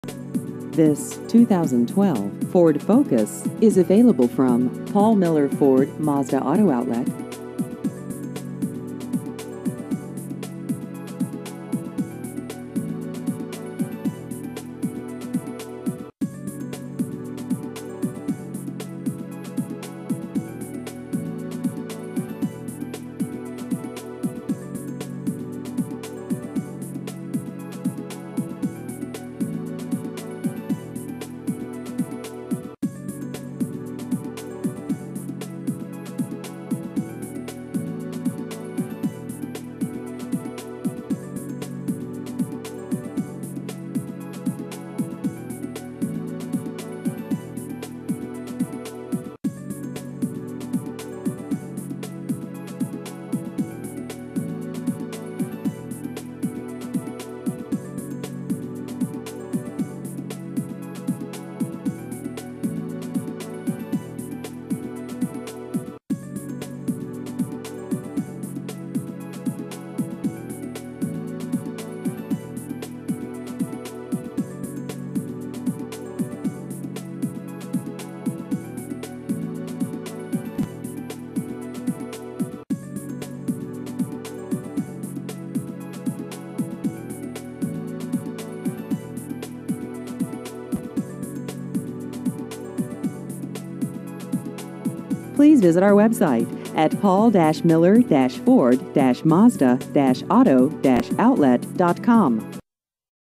2012 Ford Focus New Cars Lexington KY
This 2012 Ford Focus is available from Paul Miller Ford, Mazda, Auto Outlet.